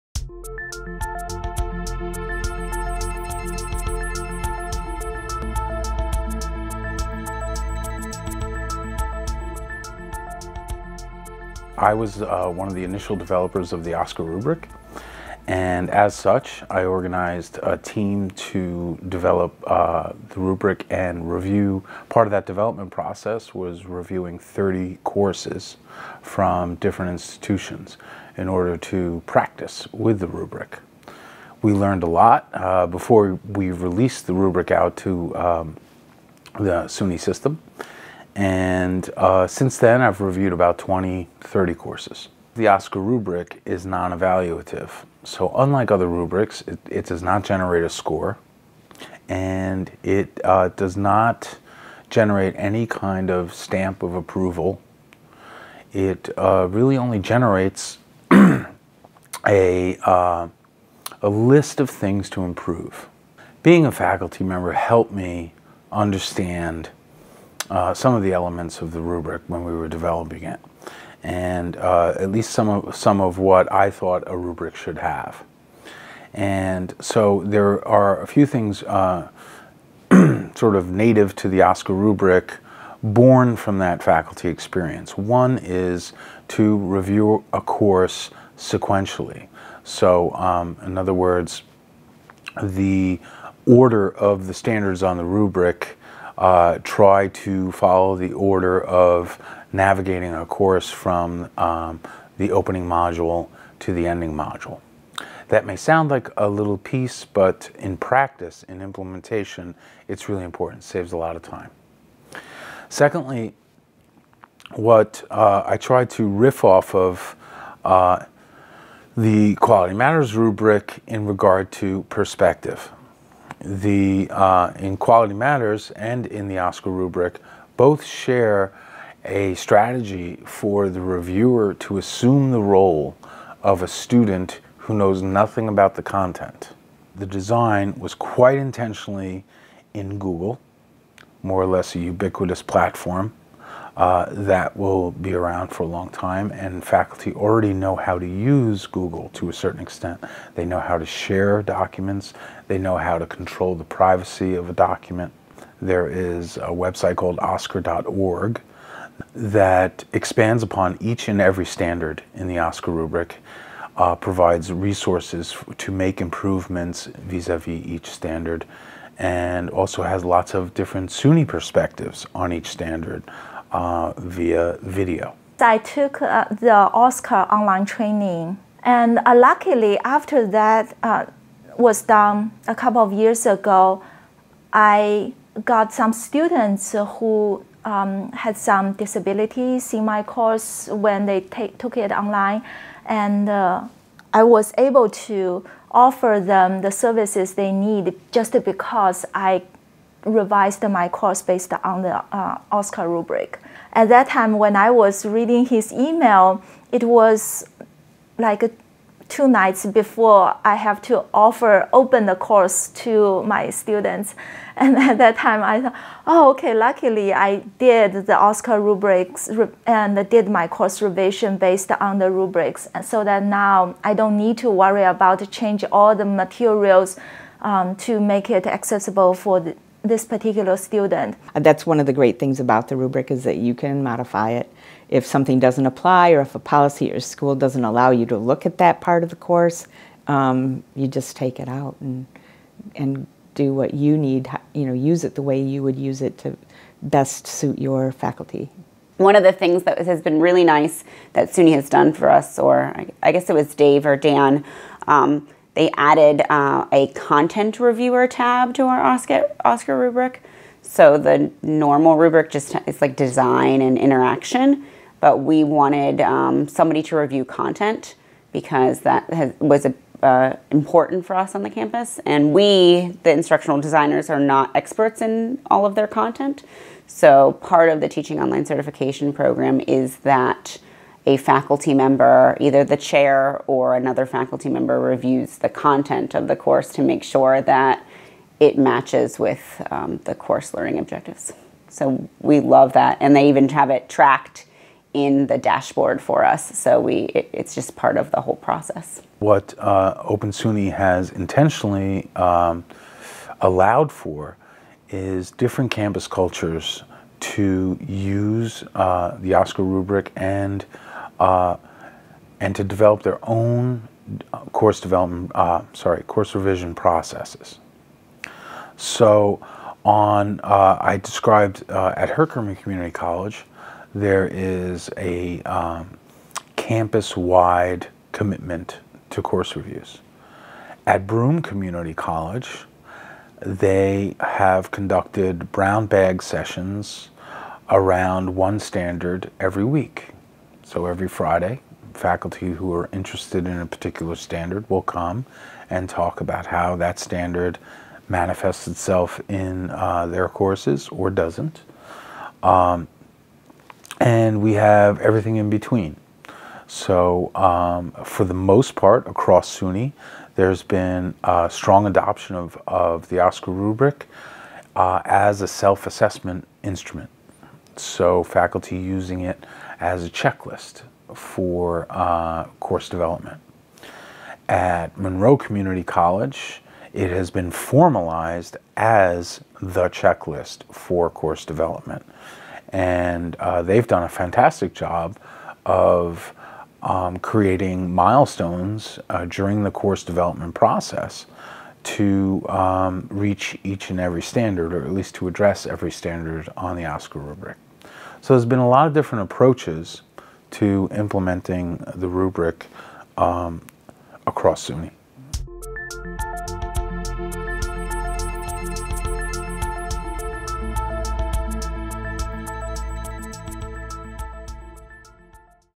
The OSCQR Rubric - Its History and Nature
Learn about the history of the OSCQR rubric and the characteristics that make it an innovative and unique approach to reviewing your online course.